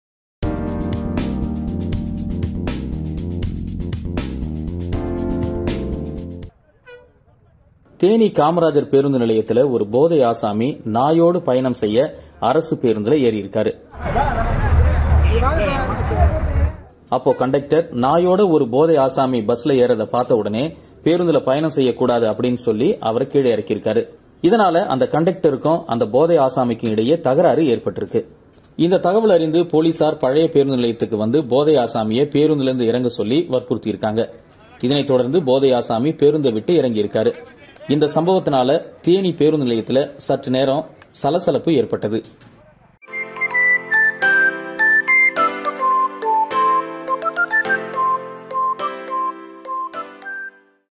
நாயுடன் பேருந்தில் பயணம் செய்த போதை ஆசாமி | Inebriated man tries to get on bus with a dog
தேனி காமராஜர் பேருந்து நிலையத்தில் ஒரு போதை ஆசாமி நாயோடு பயணம் செய்ய அரசு பேருந்தில் ஏறினார். அப்போது கண்டக்டர் நாயுடன் பேருந்தில் பயணம் செய்யக்கூடாது என்று கூற கண்டக்டருக்கும் போதை ஆசாமிக்கும் இடையே தகராறு ஏற்பட்டது.

A fight broke out between a bus conductor and an inebriated man, who attempted to board a bus along with a dog, at Theni old bus stand.